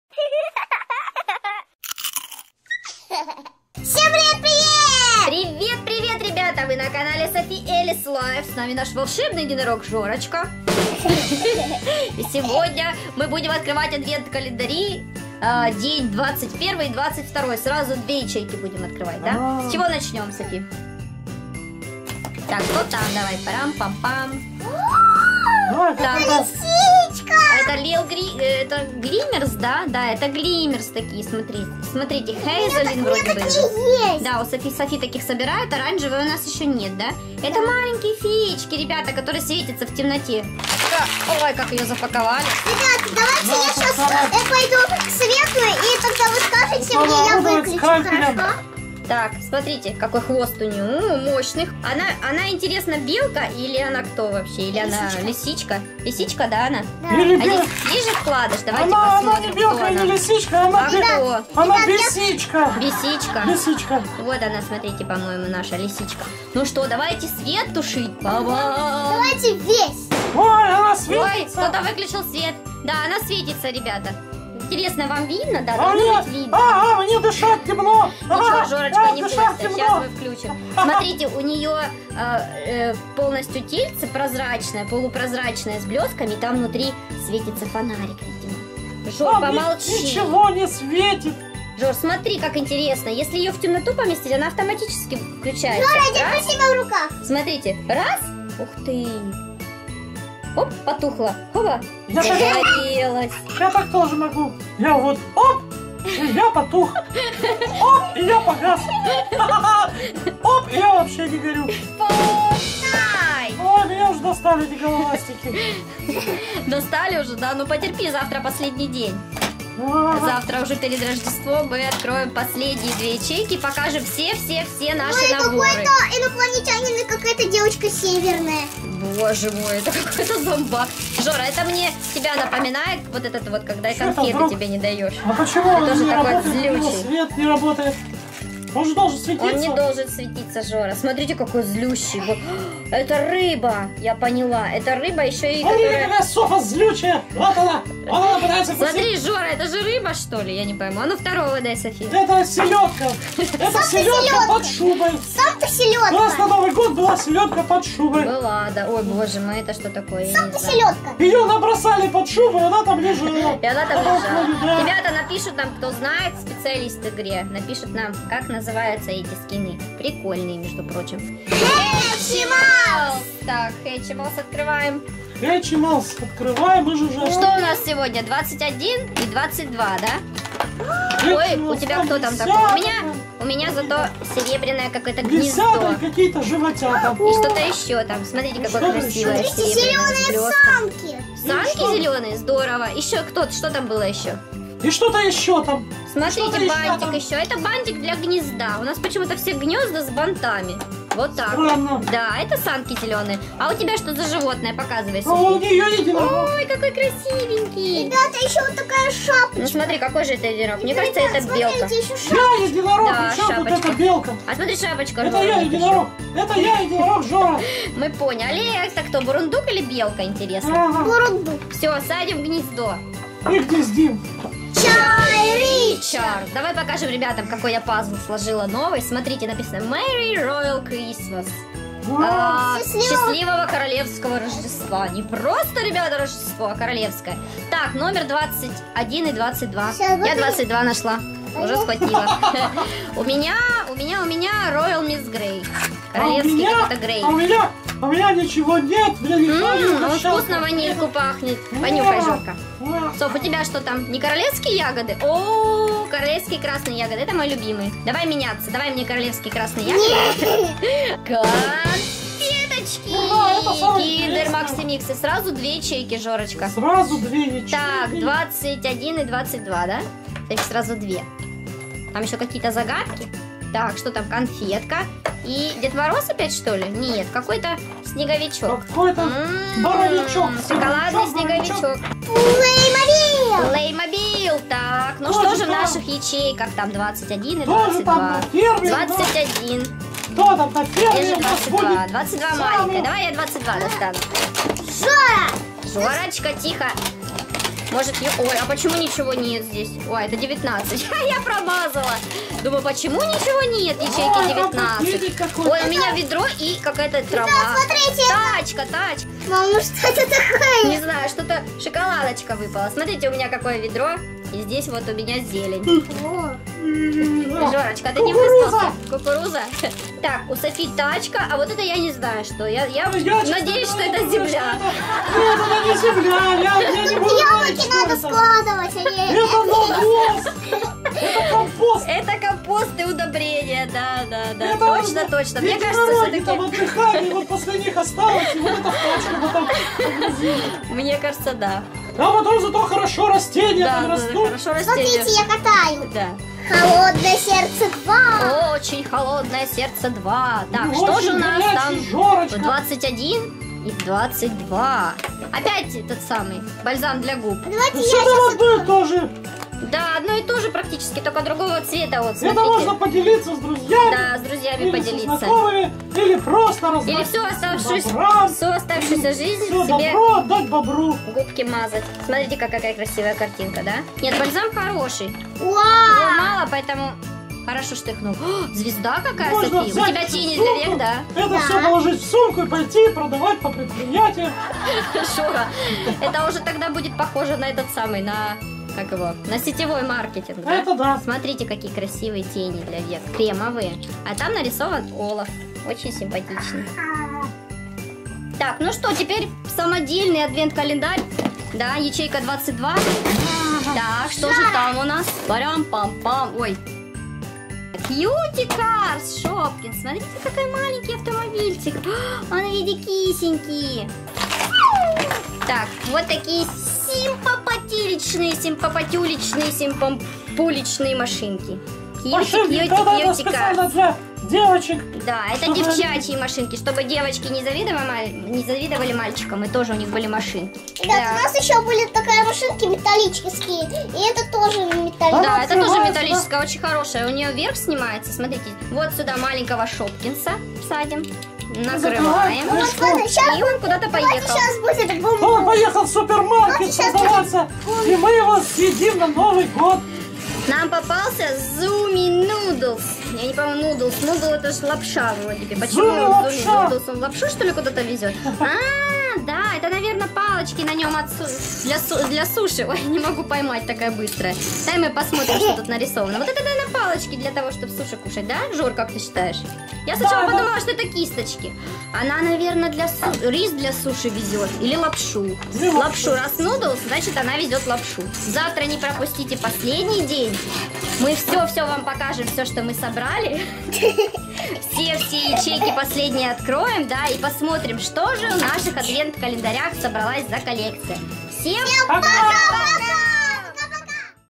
Светятся ли единороги в темноте?!! БИТВА АДВЕНТ КАЛЕНДАРЕЙ День 21-22
Поддержать наш канал и на конфеты для единорога Жоры - Карта Сбербанка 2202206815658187
День двадцать первый и двадцать второй. Сегодня мы узнаем, умеет ли наш волшебный единорог светиться, как фея! Жора и Софи-Элис открывают много оригинальных новогодних адвент календарей и один огромный самодельный! Что круче?! Адвент календарь Funko Fortnite, Hatchimals, Frozen 2, Kinder Maxi Mix, Playmobil или самодельный, который я сделала своими руками в подарок? Косметика, сладости, игрушки, сюрпризы или конструктор? Давайте посмотрим!